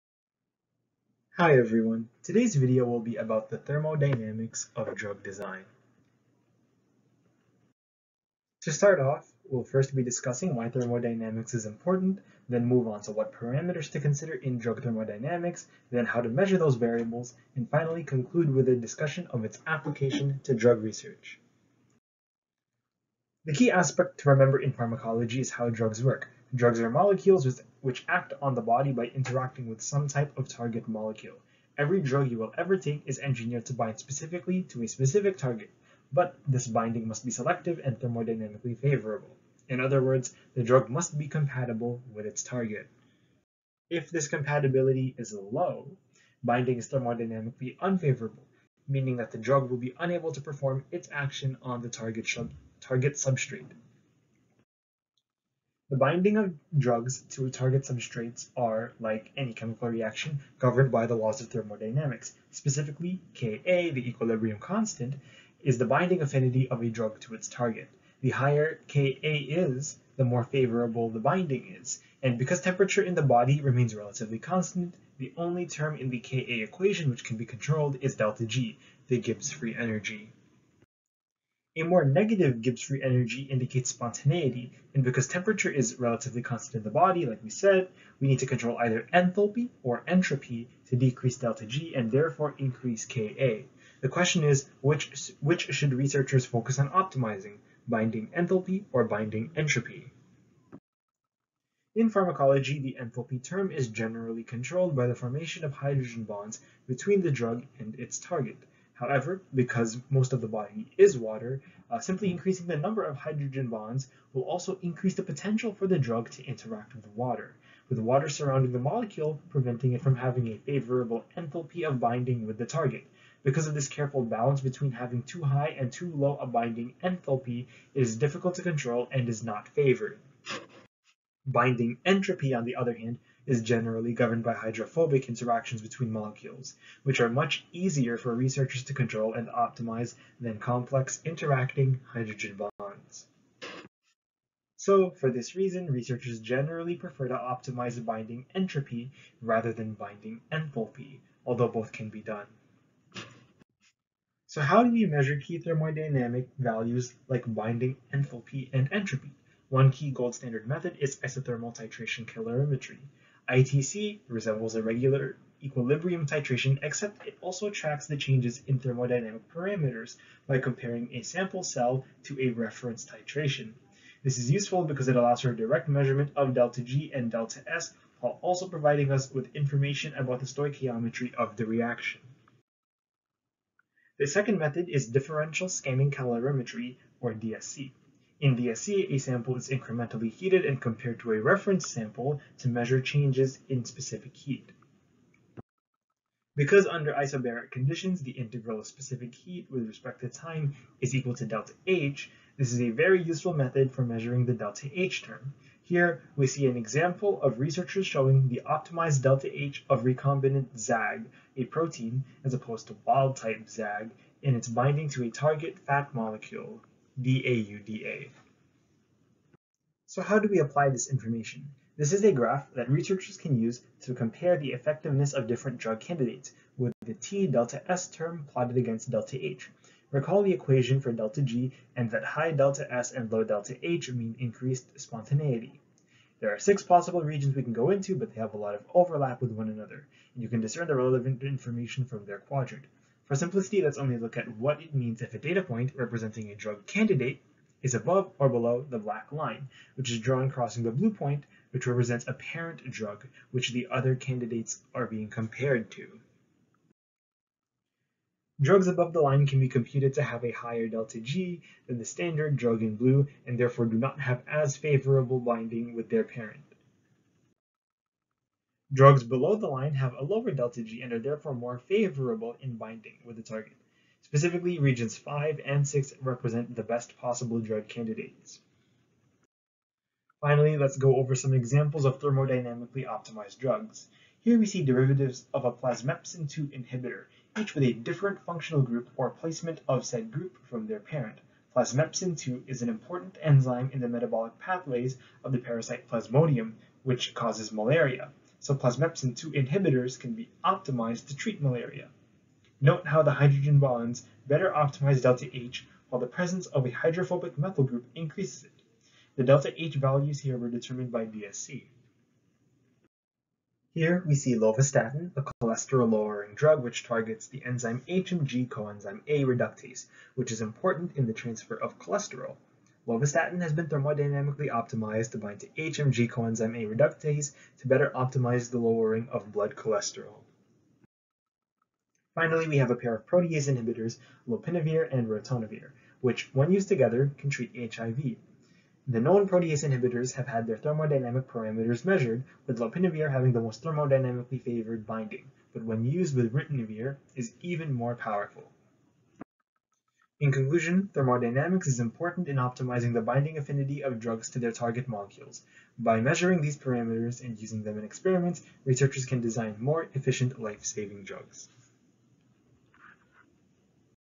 The Thermodynamics of Drug Design
This was my final project for my thermodynamics class. I made this a month ago but now that grades are officially done I'm uploading this now.

I hated this class so much.

Bruylants, Gilles & Wouters, Johan & Michaux, Catherine. (2005). Differential Scanning Calorimetry in Life Science: Thermodynamics, Stability, Molecular Recognition and Application in Drug Design. Current medicinal chemistry. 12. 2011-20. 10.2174/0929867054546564.
Freire E. (2008). Do enthalpy and entropy distinguish first in class from best in class? Drug discovery today, 13(19-20), 869–874.
Freire E. (2009). A thermodynamic approach to the affinity optimization of drug candidates. Chemical biology & drug design, 74(5), 468–472.